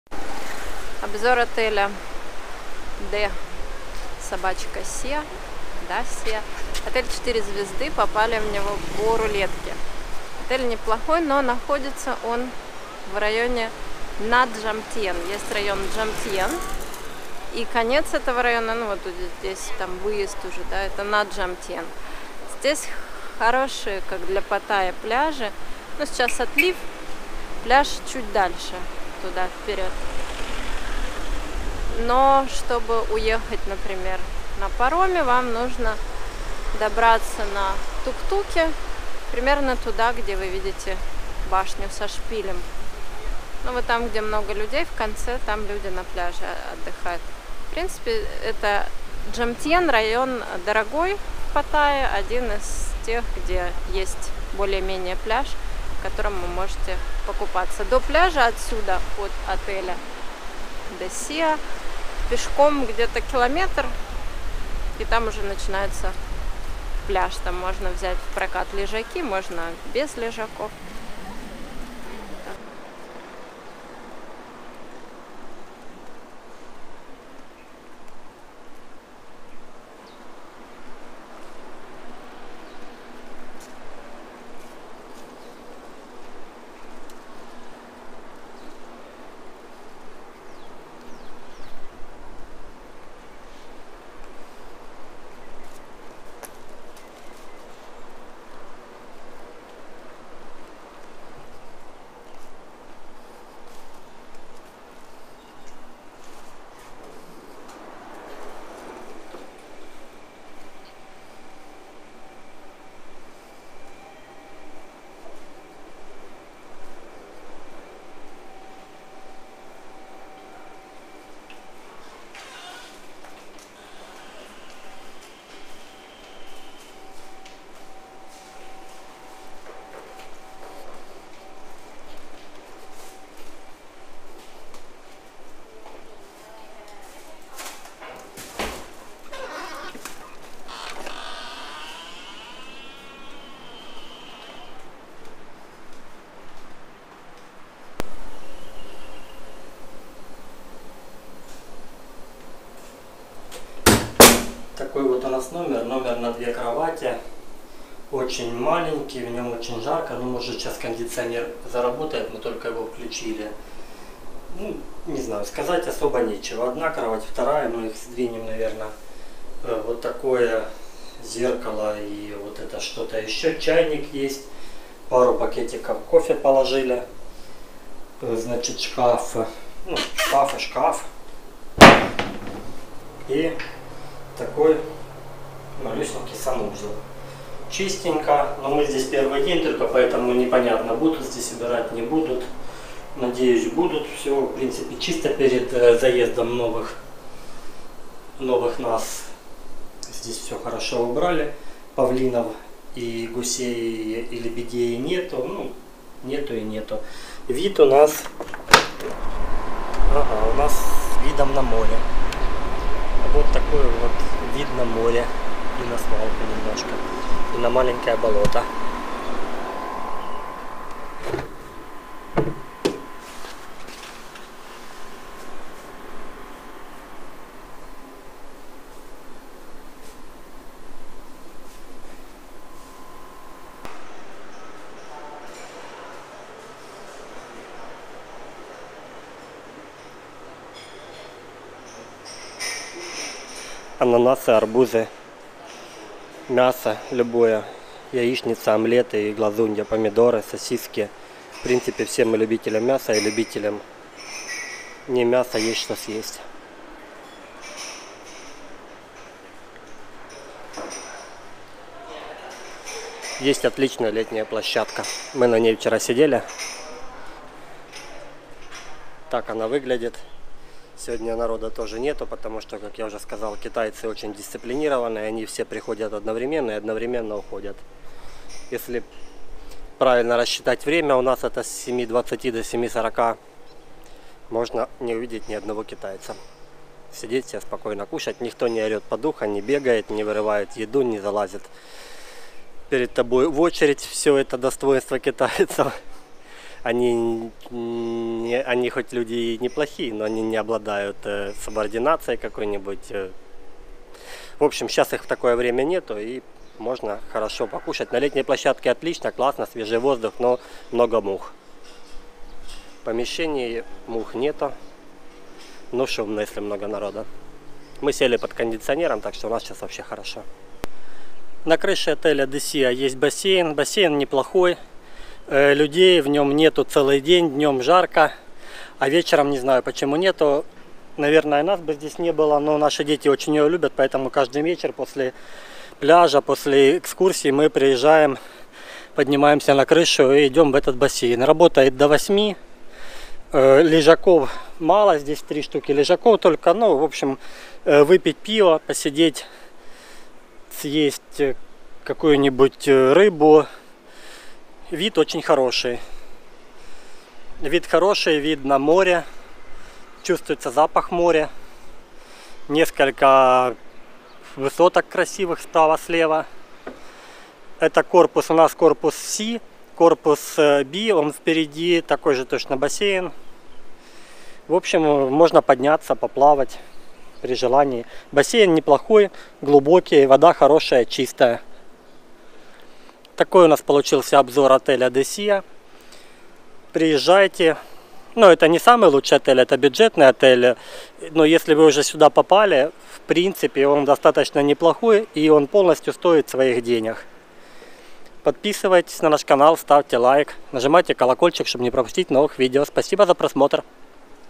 D@sea hotel pattaya | Обзор отеля в Паттайе
D@sea hotel pattaya | Обзор отеля D@sea  в Тайланде в Паттайе
↓Развернуть меня↓

Отель D@sea hotel pattaya расположен в районе пляжа Джомтьен, всего в 200 м от моря. Отель - стандартная хорошая тройка, современно оформленные, комфортные номера, открытый бассейн на крыше отеля и небольшой тренажерный зал. На первом этаже отеля ресторан, в котором можно нормально поесть. D@sea hotel pattaya расположен в районе пляжа Джомтьен, всего в 3 минутах ходьбы от моря.
Открыт в 2015 году. Мы выбрали этот отель исключительно из-за низкой цены. От отеля днем можно уехать на тук туке в любую точку Паттайи, но вечером не каждый тук-тук довезет вас обратно. Они сворачивают от береговой лини влево, на перекрестке, который в полутора километрах от D@sea hotel pattaya. Дальше придётся пешком. В целом, это не напрягало, но если бы мы выбирали отель сейчас, то искали бы по ближе к ночному рынку. Наше пребывание в отеле D@sea hotel pattaya выглядело приблизительно так:
1) Раннее утро. Завтрак
2) Утро-обед - Экскурсии/пляж
3) Вечер - Ночной рынок/прогулка вдоль пляжной линии
4) Поздний вечер - Бассейн на крыше отеля
D@sea hotel pattaya состоит из двух 7-этажных зданий с 229 номерами. Если ищите бюджетный вариант в Паттае - D@sea hotel pattaya достоен рассмотрения.

НАШИ САЙТЫ И ПРОЕКТЫ